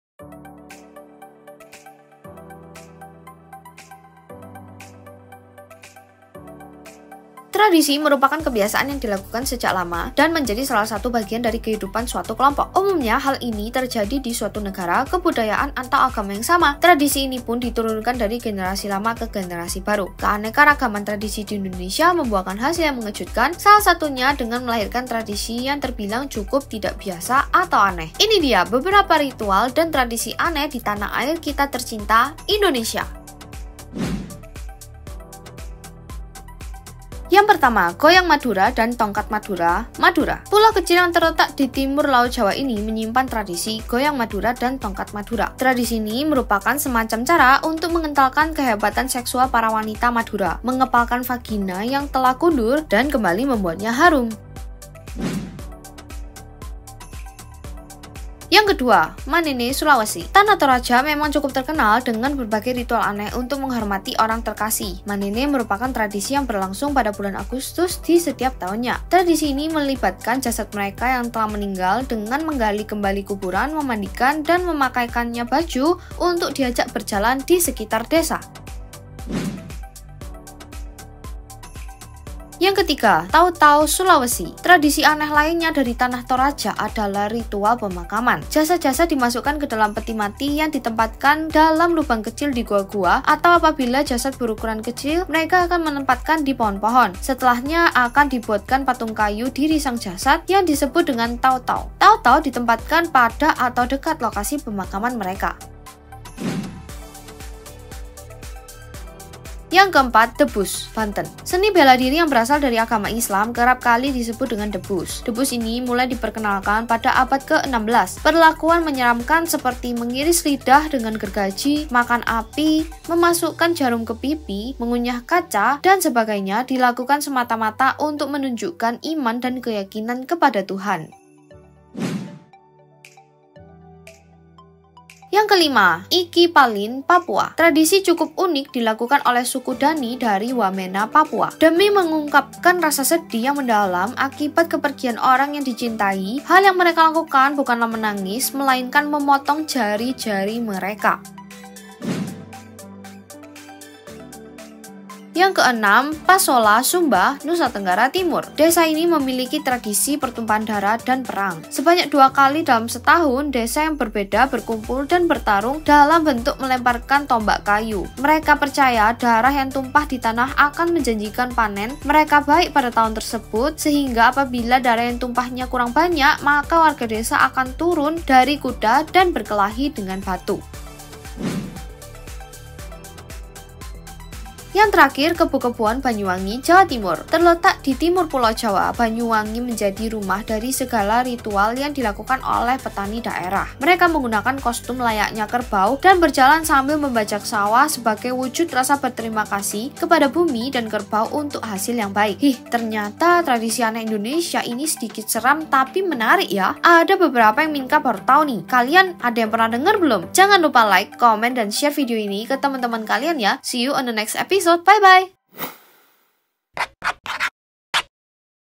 [IS FAKTA eps.49] Fakta Tradisi Unik Dari Indonesia
Keanekaragaman tradisi di Indonesia membuahkan hasil yang mengejutkan, salah satunya dengan melahirkan tradisi yang terbilang cukup tidak biasa.
Ini dia beberapa ritual dan tradisi aneh di tanah air tercinta kita Indonesia... Penasaran tradisi apa aja?? Langsung aja simak video nya!!

Bila kalian ada topik yang ingin mimin bahas untuk isFakta, kalian bisa komen dibawah ya!!!